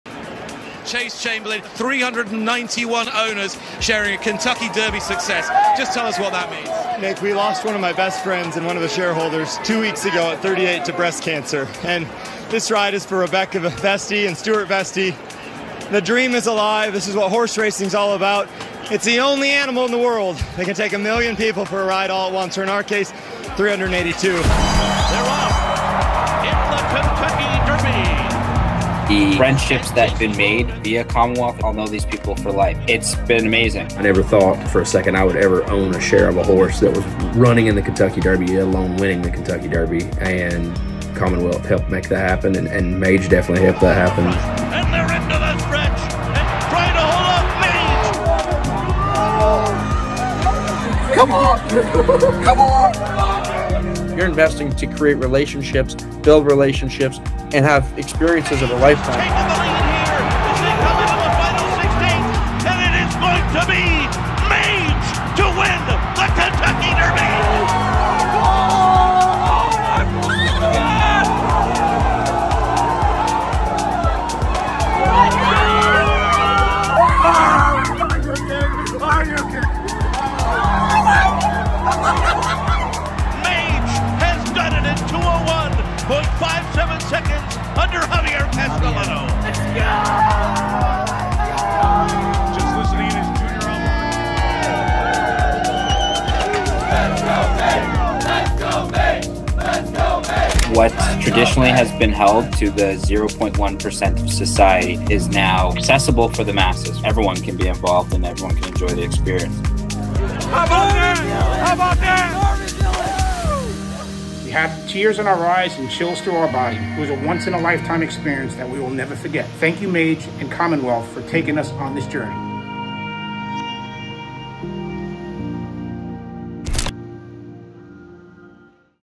2023 Kentucky Derby - CMNWLTH NATION
It’s been a year since the 149th Kentucky Derby, and one thing stands out: while great horses are rare, it’s the people we share these moments with who truly matter. CMWNTLH Nation will always cherish May 6th, 2023. We’re in this together!